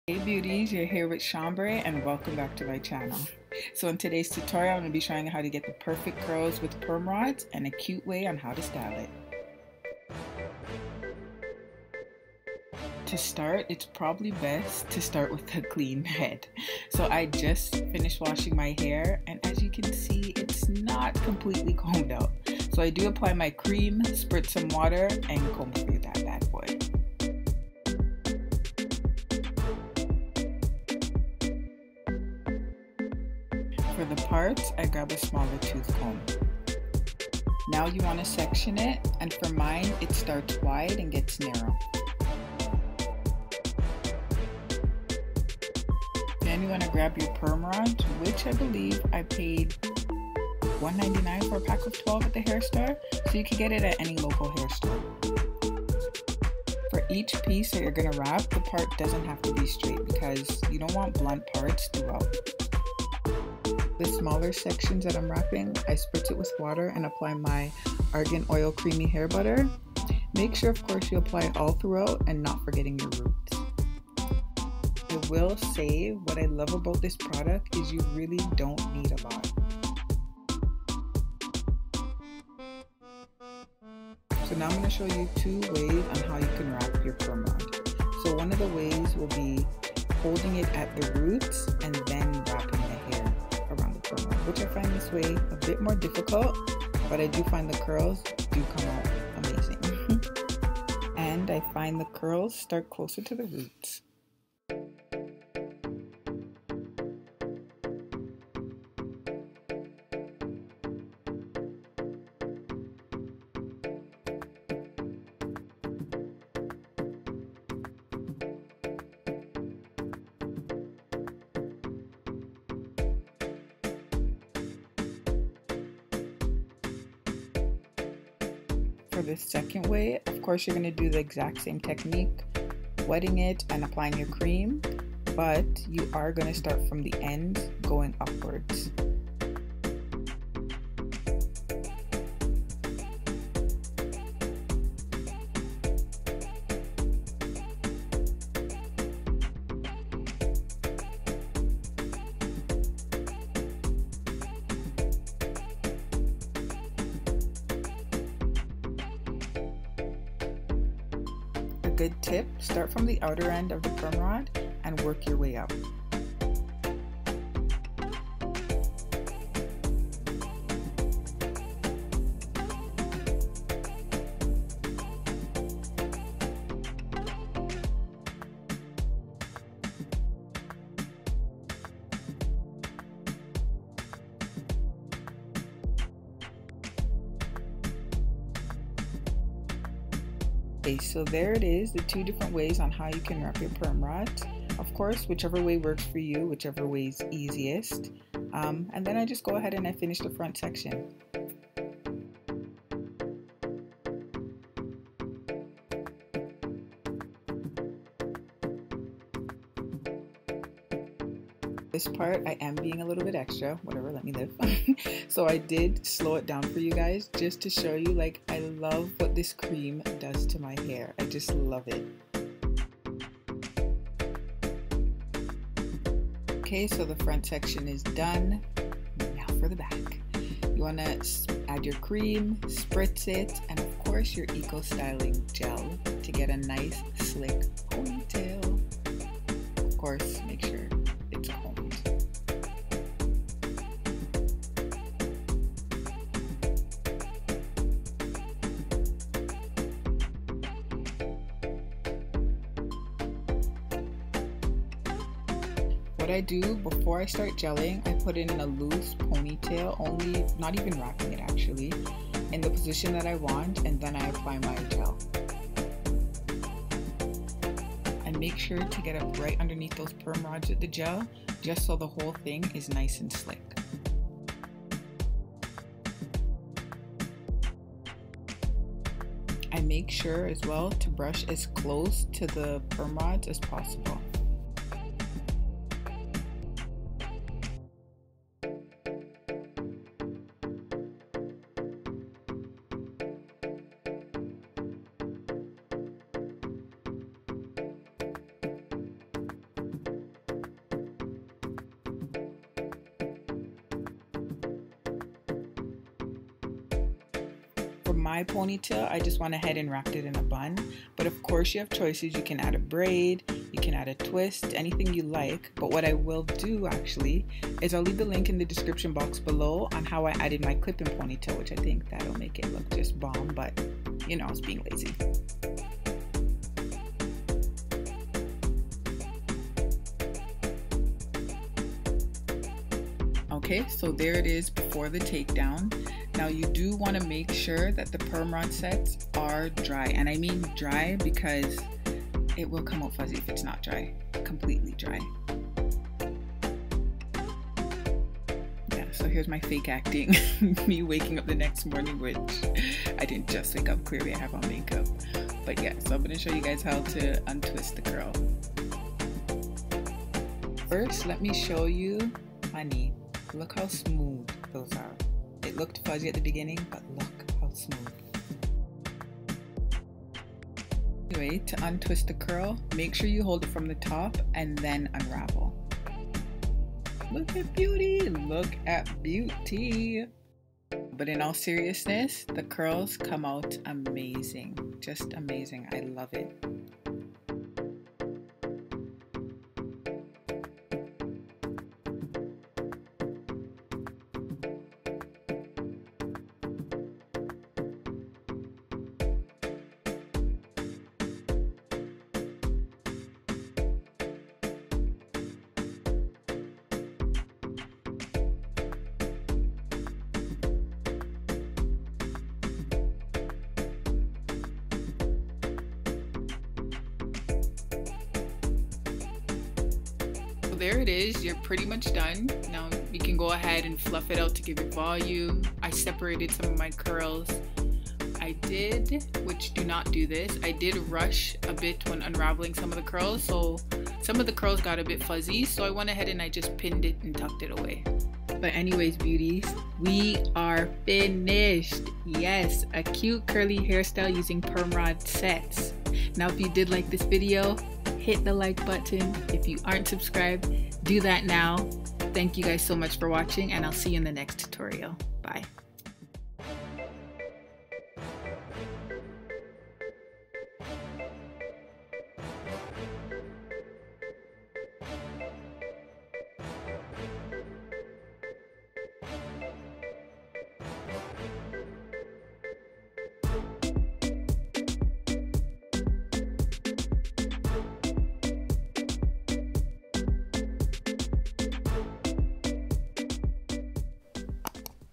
HOW TO | Perm Rods on Natural Hair x Styling
Hey My Lovelies. The quickest and easiest Permrods technique for all hair types, especially those with with thick, frizzy hair. Give it a try if you're not usually a fan or you just haven't had success with them yet! You’ll love it!
As promised in the video 👇🏽

Other C U T E • H A I R  • T U T O R I A L S